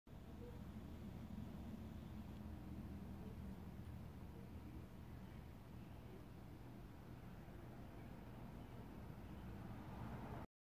Head Lice through a microscope. Take 1.
Head Lice through a microscope. Placed on an open glass slide. Take 1.

Estimated to be 1/4 to 1/2 the size of an adult previously found before treatment.

48 hours after treatment with Perethrin.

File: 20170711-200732